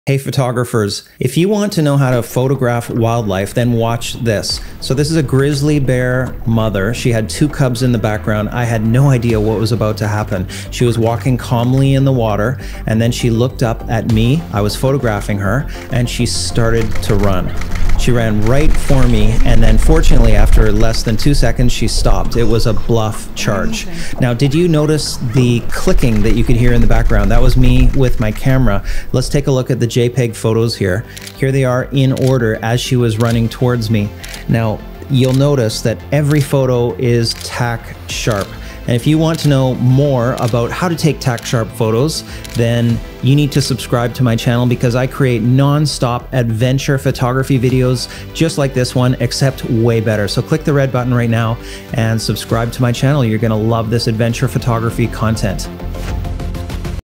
How to photograph a running grizzly bear
This is how I photographed a running grizzly bear in the wild.  Make sure you are using continuous focus and not single focus.   Also, you want to use back button focus and of course, rapid-fire.  I was shooting with a Nikon D850 in continuous shooting mode with a 500mm prime lens.  The video was taken with a Sony a6400.

This video was made for the new Youtube Shorts content and the video length can only be 60 seconds.